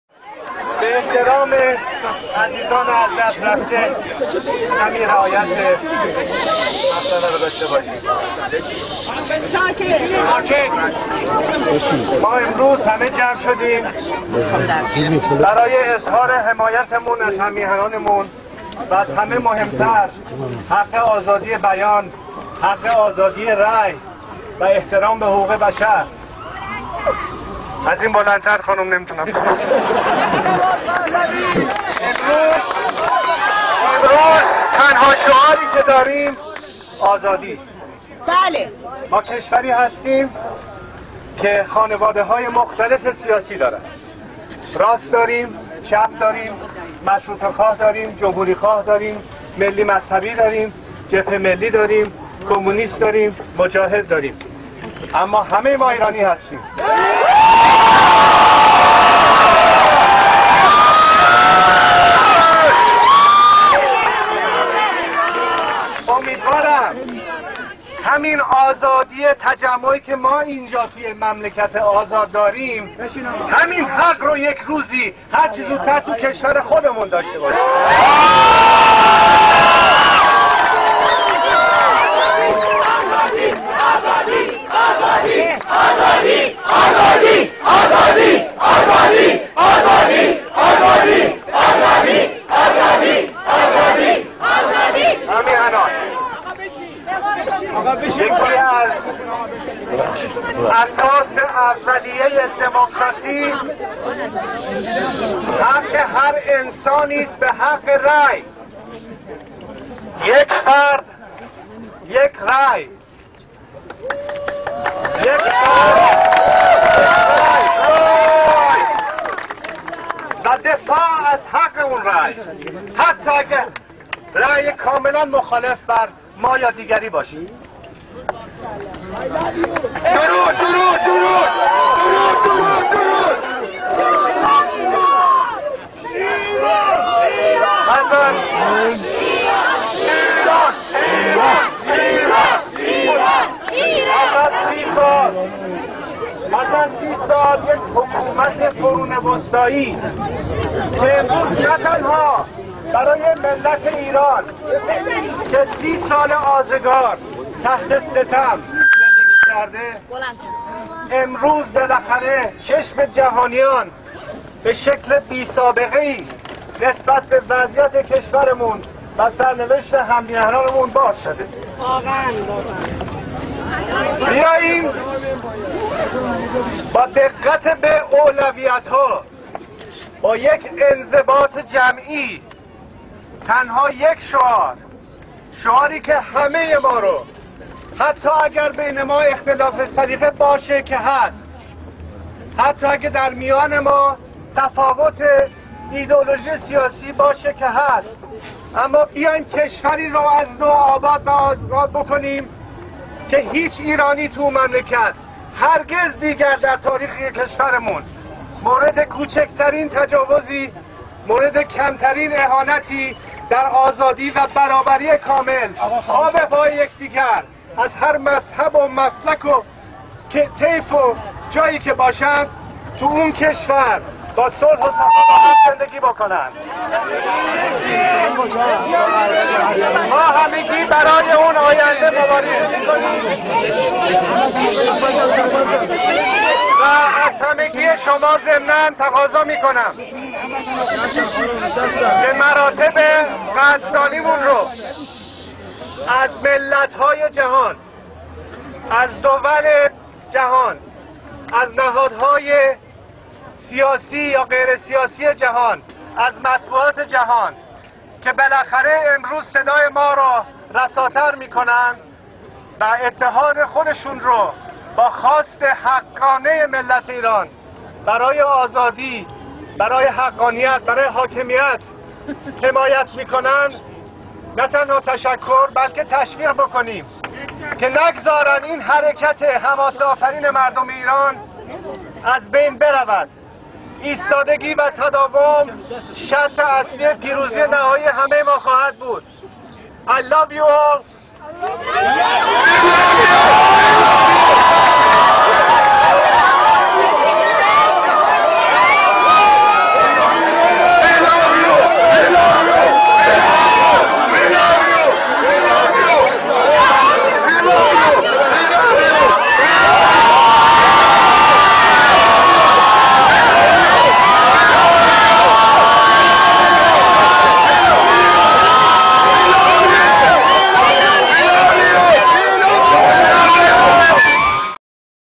Reza Pahlavi of Iran at White House Democracy Rally June 21, 2009
Reza Pahlavi speaks to pro-democracy protestors at the White House, Washington, D.C., on June 21, 2009, calling for Iranians to unite.
The Secretariat of Reza Pahlavi of Iran salutes IceScenic, a worthy son of Iran, for production of this video and for kindly granting permission for use of this video on this page.